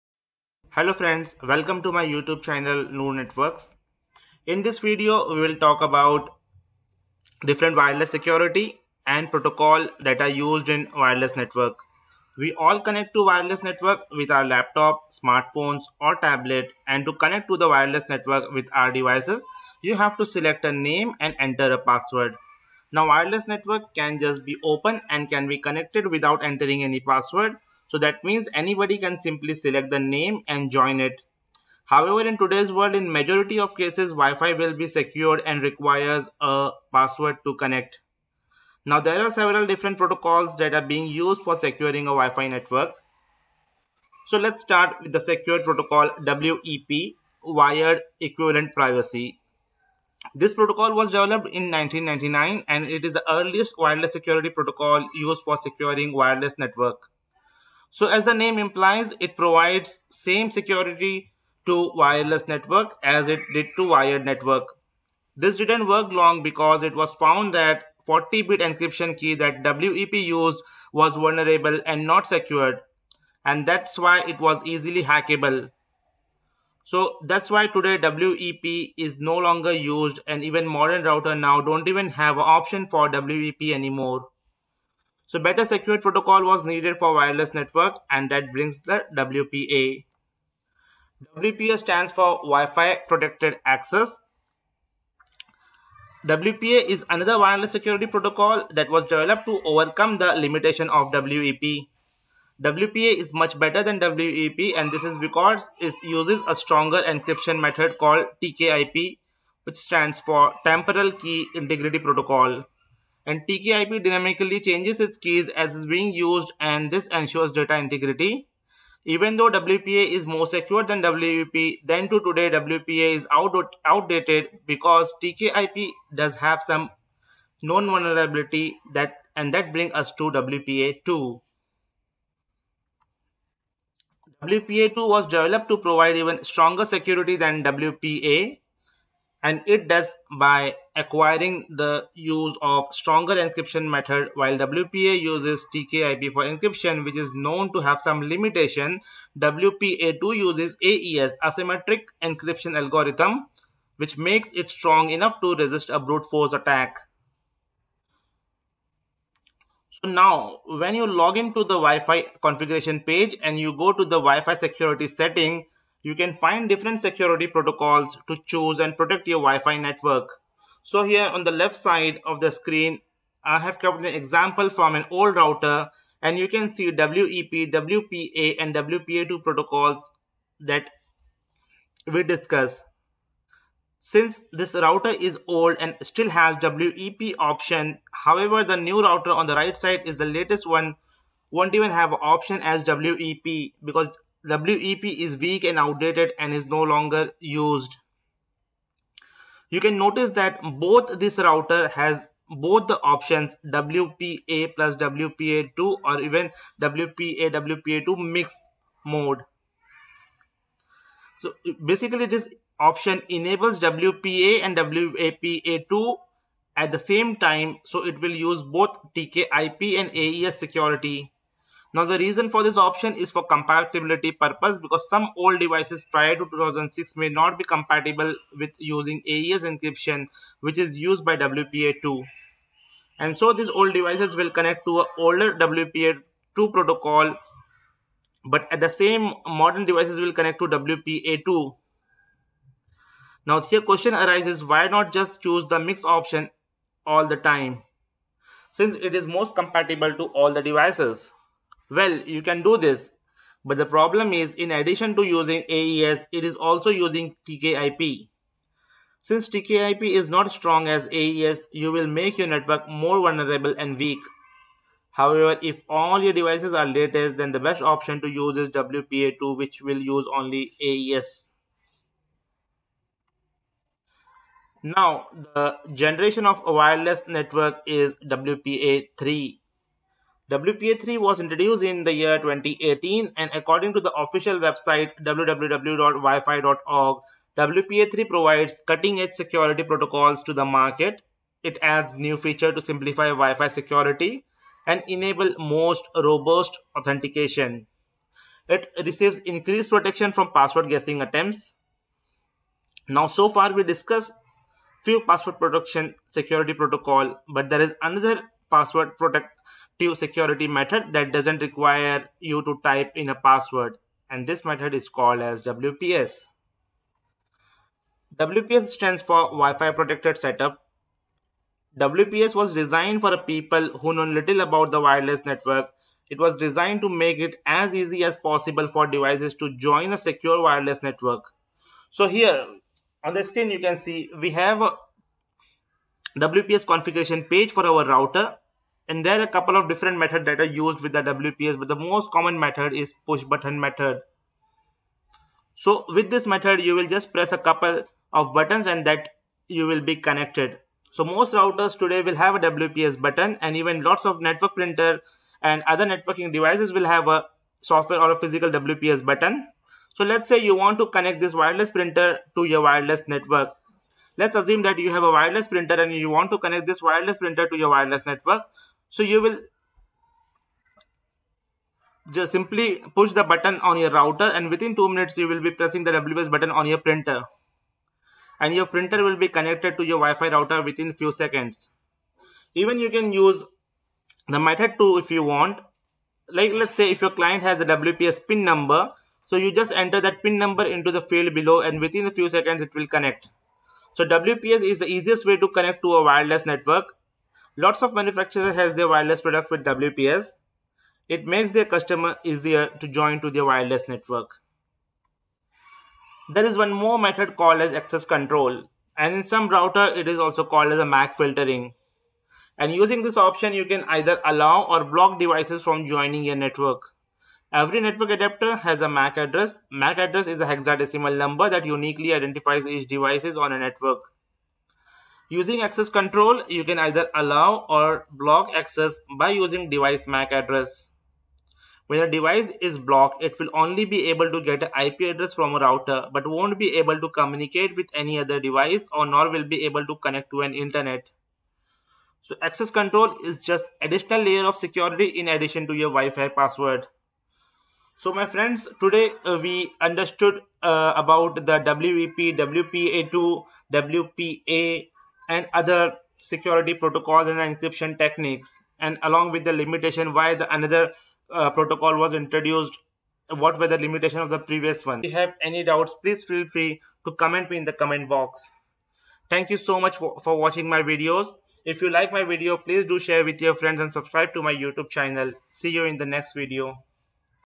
Wi-Fi Wireless Password Security Protocols
In this video we will see about wireless password security options.

Understanding different type of security modes - WEP, WPA, WPA2, WPA3, WPS, and Access Control.  Understanding of Encryption Techniques - AES and TKIP.